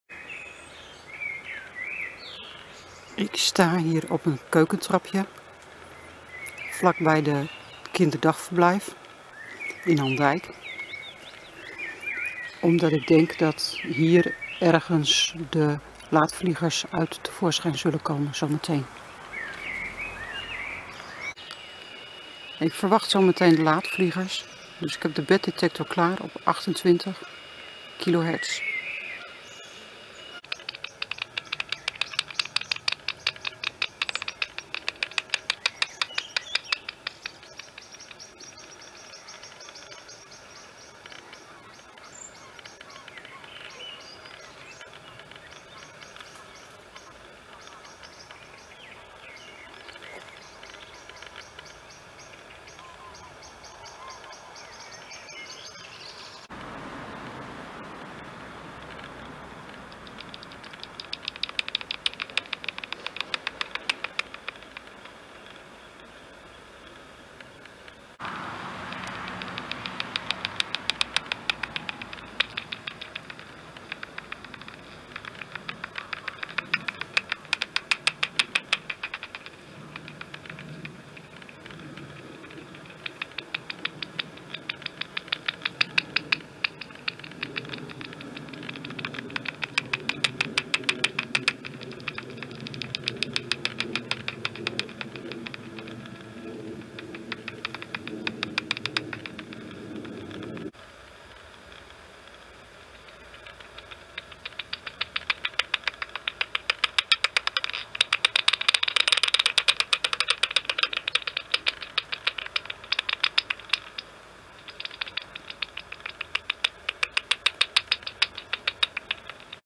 Laatvliegers in Andijk 7 mei 2023
Vleermuizen spotten op een mooie avond, met geluid van de batdetector. Een paar dagen eerder waren er minstens 8 Laatvliegers, op 7 mei nog maar 3 of 4, daarna hadden ze deze plek verlaten. Begin juni zaten ze een week lang iets verderop, in de Steenpoortestraat.